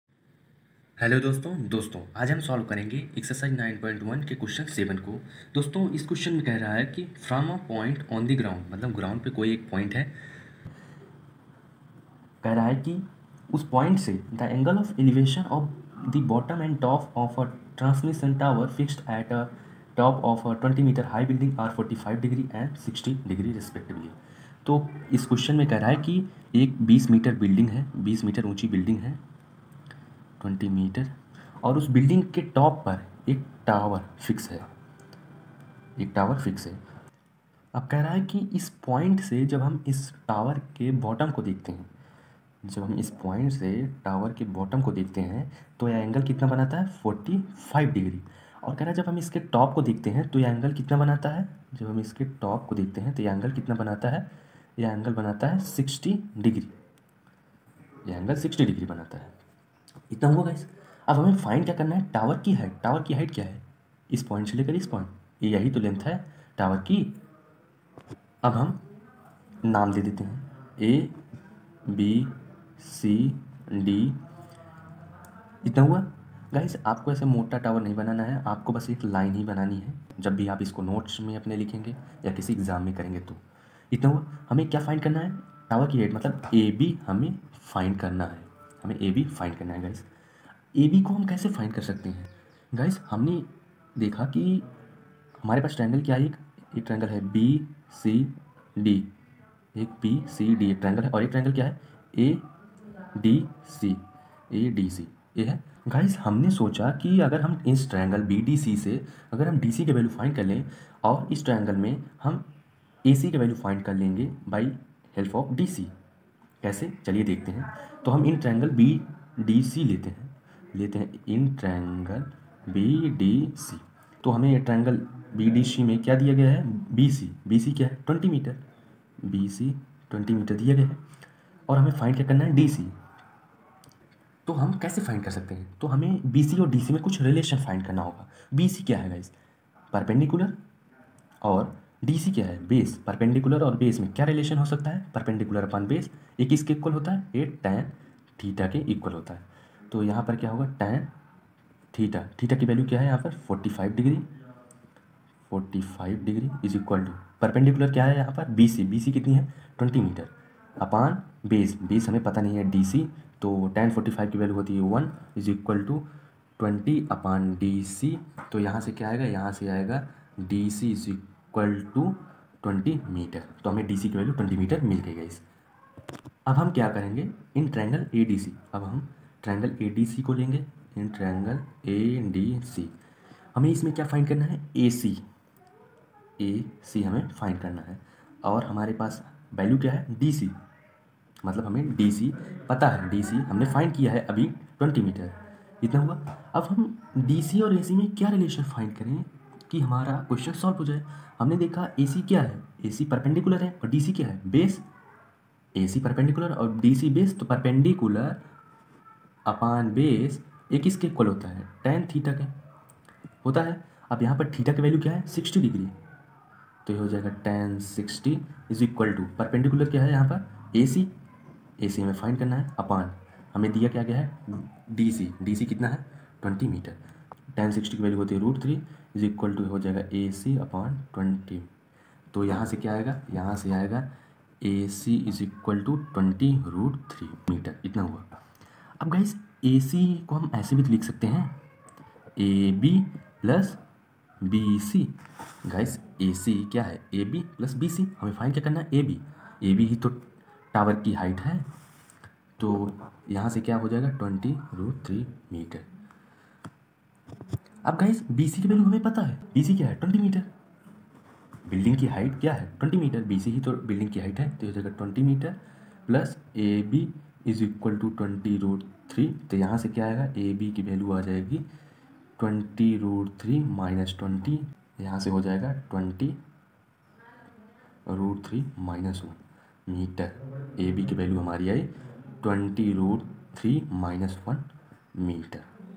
Question 7 Exercise 9.1 Class 10 Chapter 9 Applications of Trigonometry Class 10 Maths |Study Wallah
About this Video 👇
IN THIS VIDEO I HAVE EXPLAINED THE EXERCISE 9.1 QUESTION - 7 OF CHAPTER 9 APPLICATIONS OF TRIGONOMETRY CLASS 10 MATHS.

👉➡️➡️IF YOU HAVE ANY DOUBT THEN LEAVE A COMMENT👈⬅️⬅️

( About STUDY WALLAH - Ekansh Mishra -  I will teach you full Ncert Maths Class 10 )